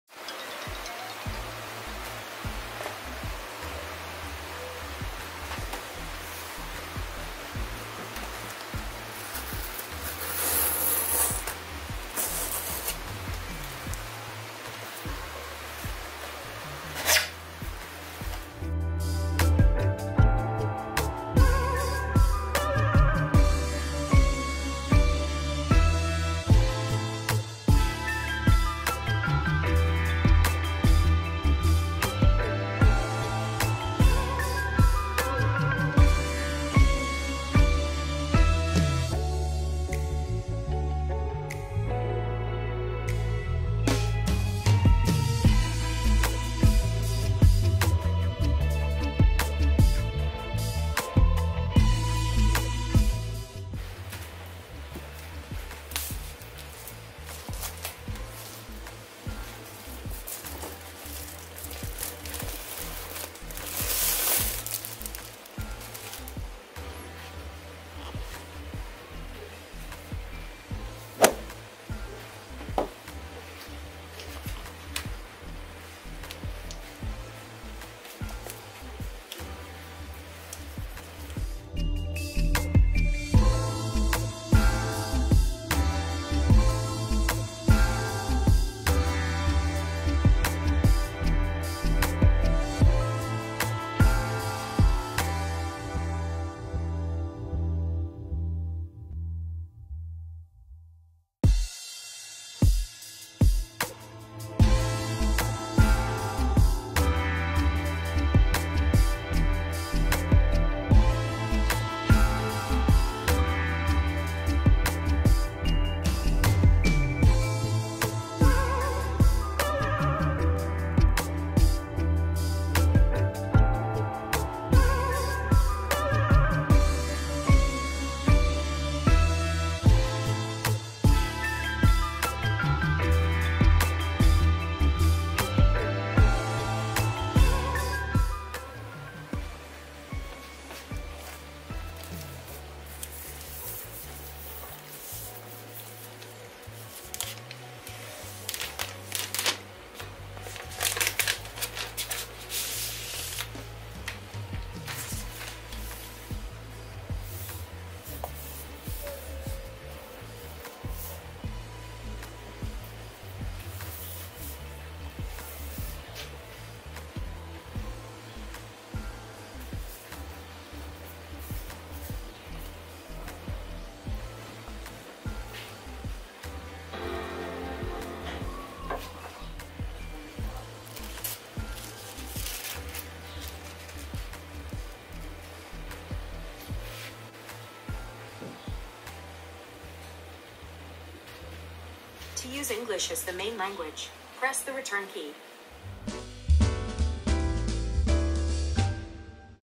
Unboxing of silver M1 MacBook Air 💻 / Philippines 🇵🇭 / My reward for working 15 straight years 😭
My 1st ever MacBook. Finally. 🥲

To be used for more content. 😊